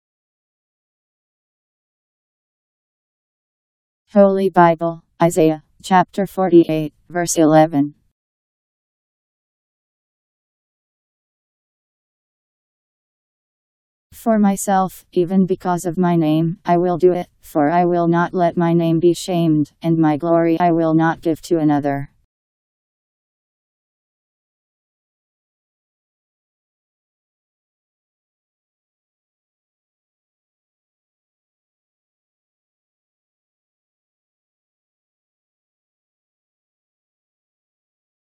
Holy Bible Isaiah 48:11
Video shows Holy Bible Isaiah Chapter 48 Verse 11: For myself, even because of my name, I will do it; for I will not let my name be shamed; and my glory I will not give to another. Powered by MaryTTS. Text taken from Bible in Basic Edition. Bible Isaiah 48:11